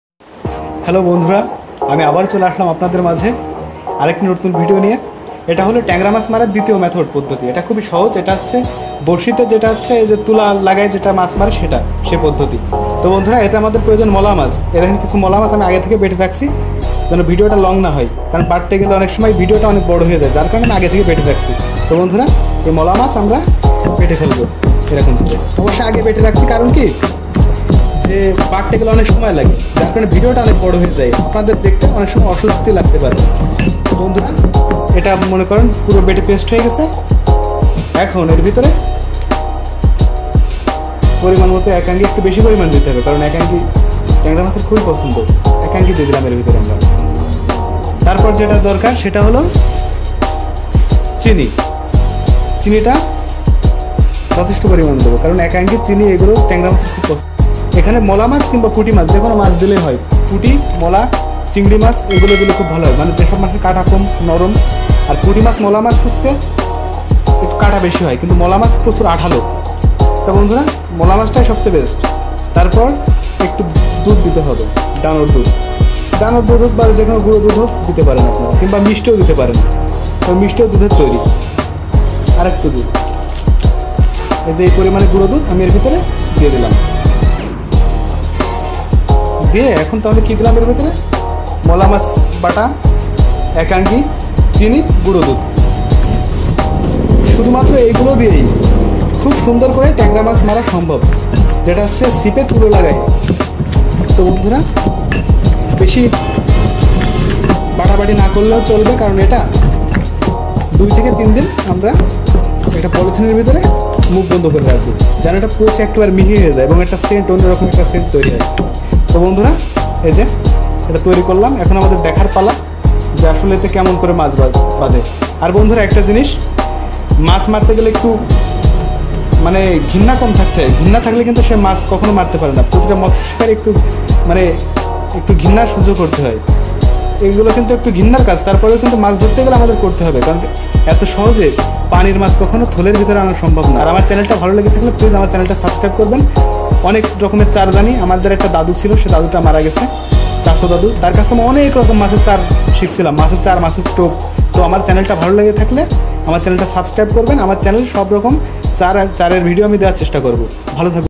ট্যাংরামাছ ধরার টোপ তৈরি | কৈ মাছ-জিওল/শিং মাছ-গুলসা মাছের টোপ Tangra Fishing Bait | togor Fishing
স্বাগতম মৎস শিকারী ভাইদের

আজ আমি আপনাদের মাঝে এমন এক্টি টোপ বানিয়ে দেখাচ্ছি যেটা ১০০% ট্যাংরা মাছের জন্য কার্যকর।
 এই টোপে ব্যবহার করা হয়েছে।
*মলা মাছ (পুটিমাছ দিলেও চলবে)
*একাংগী
*গুড়াদুধ
*চিনি
*শুটকি মাছের গুড়া ( এটা না দিলেও চলবে)

ধন্যবাদ মাছ শিকারিদের